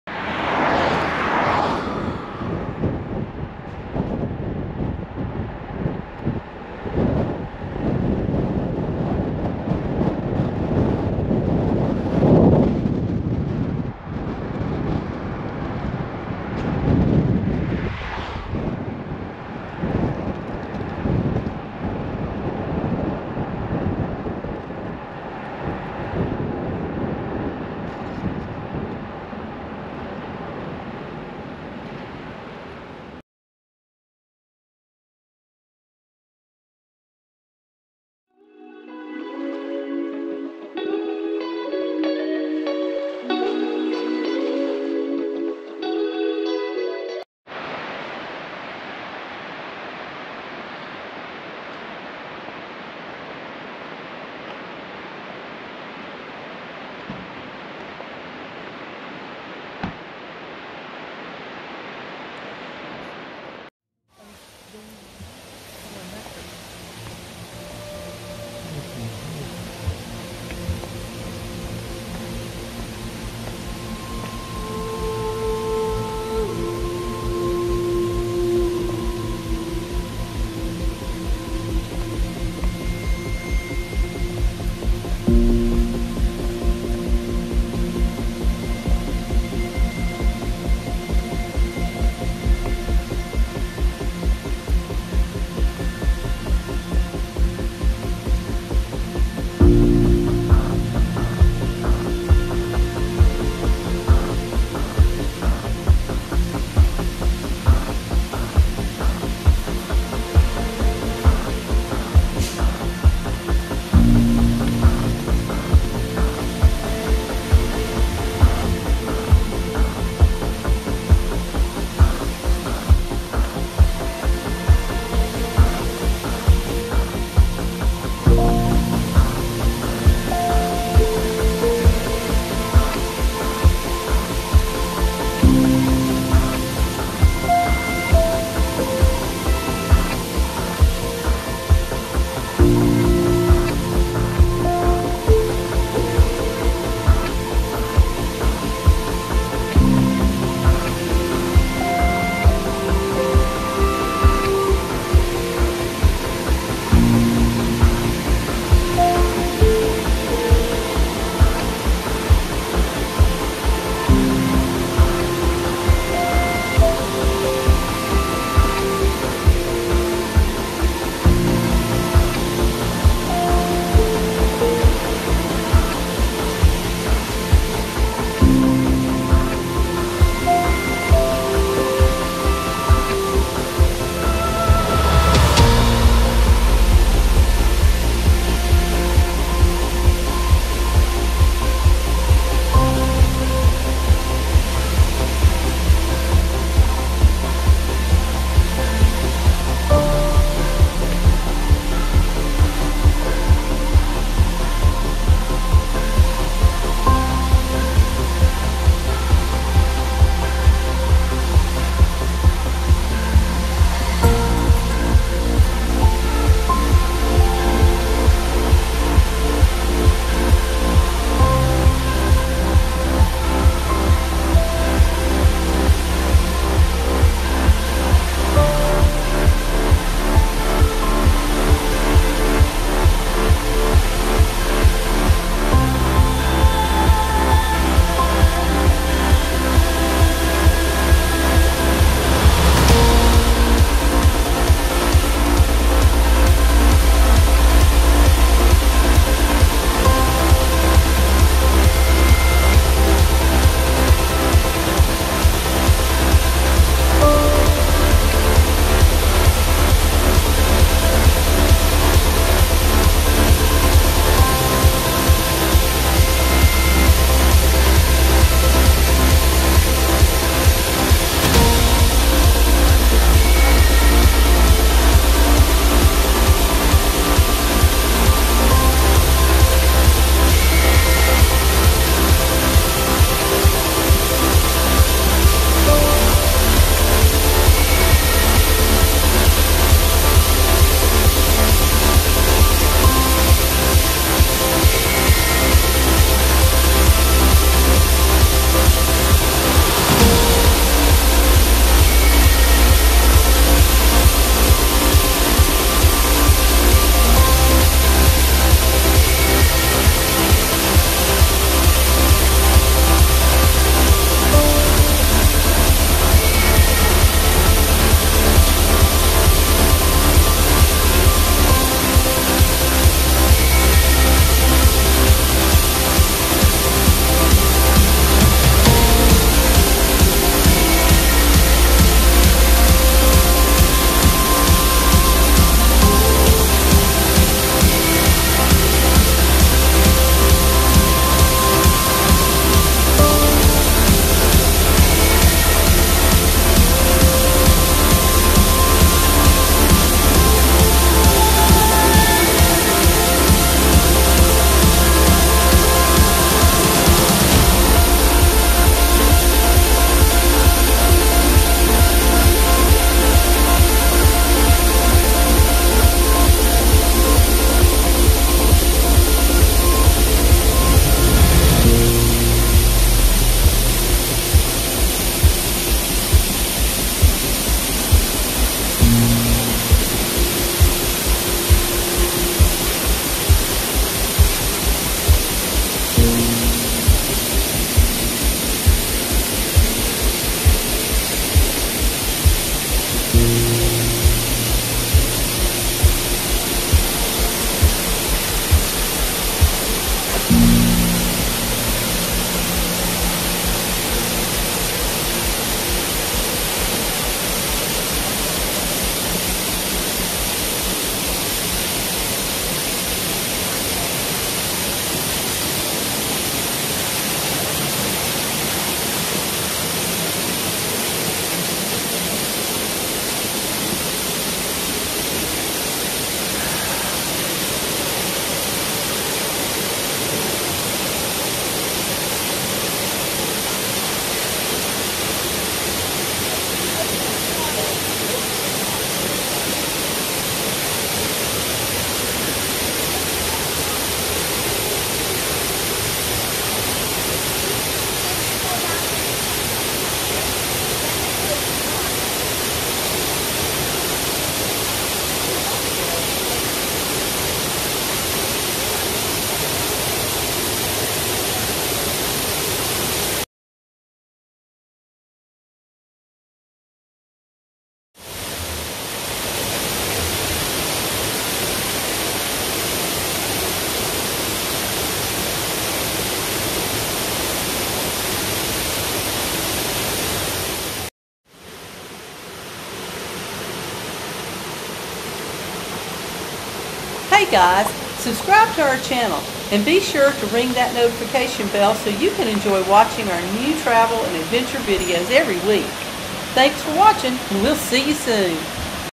Dry Falls
Dry Falls is is locatedjust off highway 64  about 3 1/2 miles west of Highlands, North Carolina. There is ample parking, a restroom and a handicap viewing platform.
You can't see the falls from the parking lot but it is a short 100 yard hike to the falls.
There are some stairs but they are well maintained and makes for an easy descent and ascent.
The most impressive part is the path goes behind the falls and the kids will love it.
Even though it is known as Dry Falls there is usually a heavy flow of water.